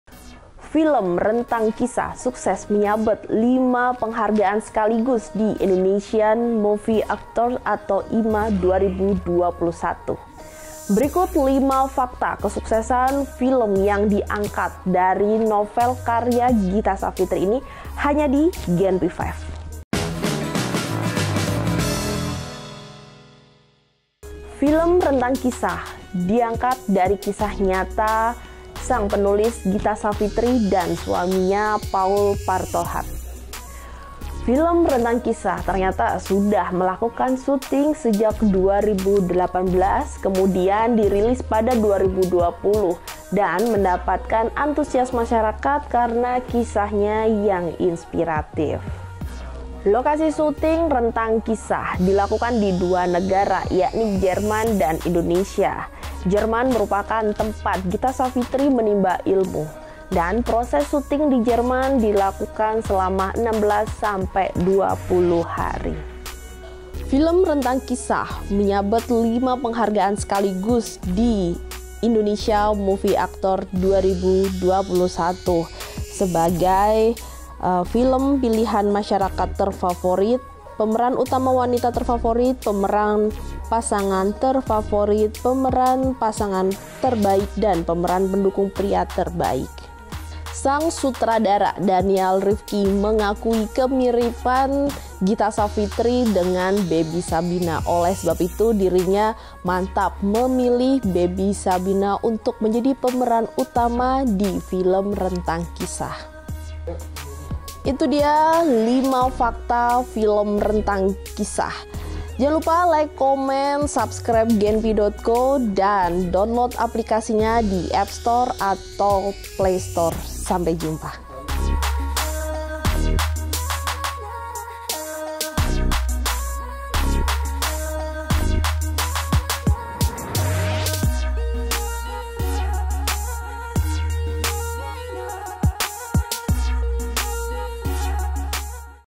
5 Fakta Film Rentang Kisah yang Nggak Banyak Orang Tahu
Film berjudul Rentang Kisah memborong penghargaan di ajang Indonesian Movie Actors Awards (IMA) 2021.   Film ini memiliki fakta menarik yang belums emua orang tahu, salah satunya proses produksi yang dilakukan sejak tahun 2018. Berikut 5 fakta menarik film Rentang Kisah pemborong penghargaan IMA 2021.

JANGAN LUPA INSTAL APLIKASI GENPI.CO :
APPSTORE:

BACA JUGA: Makam Vanessa Angel Mau Dipindah, Sang Sahabat Mendadak Emosi